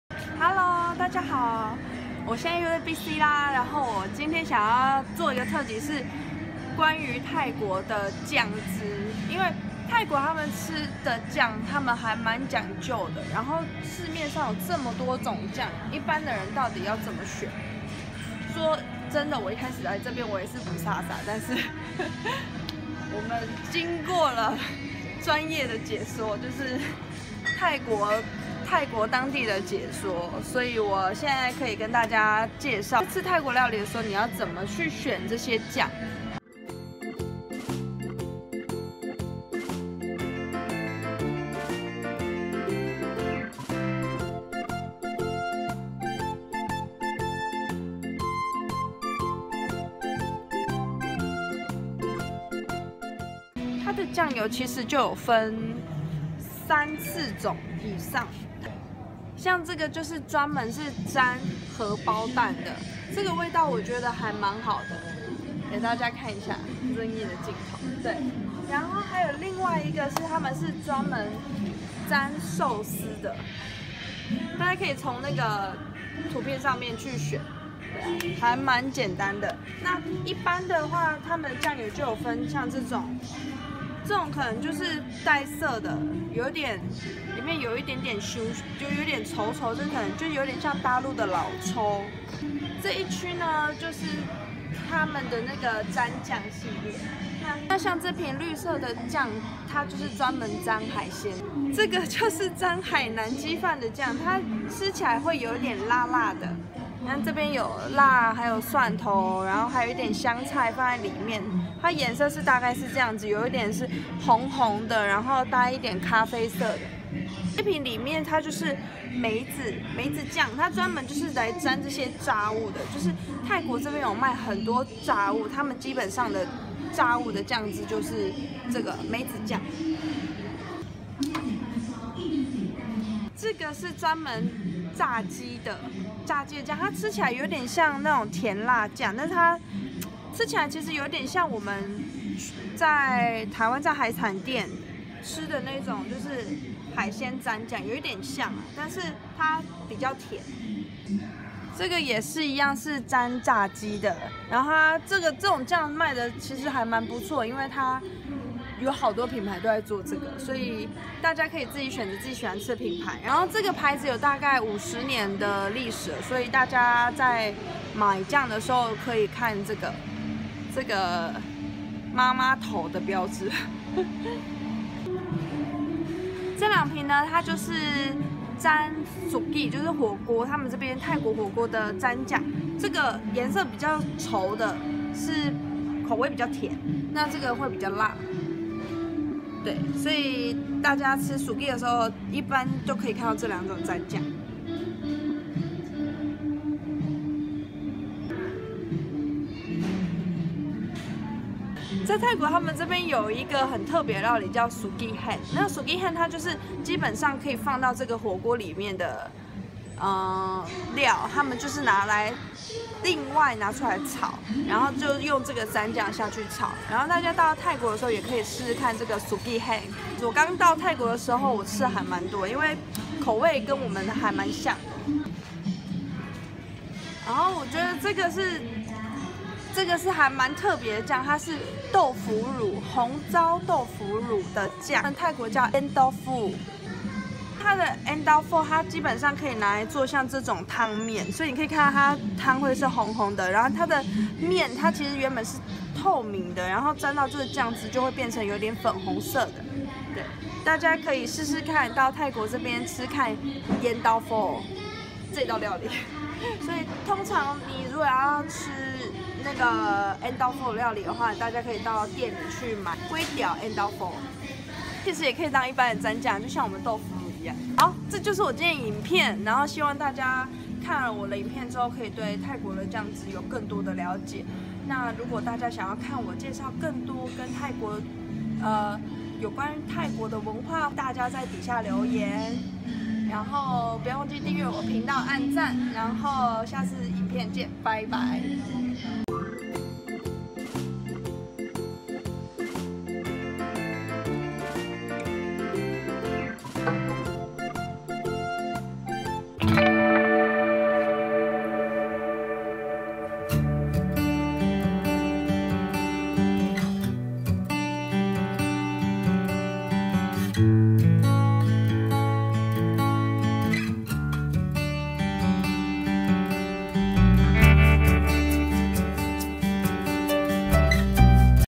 [曼谷直擊]泰國美食沾醬特集Thailand street food sauce
泰國小吃百百種，
沾醬也隨著不同料理而不同，
為了讓大家把泰國美食帶回家，
今天做了沾醬特集，
希望大家去bigc的時候，
可以買對沾醬，
把泰國美味帶回家喔

There are many type of Thai street food
Sauces are also different with dishes.
This video is about Thai sauce
if you love Thai food same as me
watching this video until the end
I will tell you what is my favorite sauce in thailand
don't forget to click the "Red Button" and you will not miss my video
Let's Bring Thai delicious food home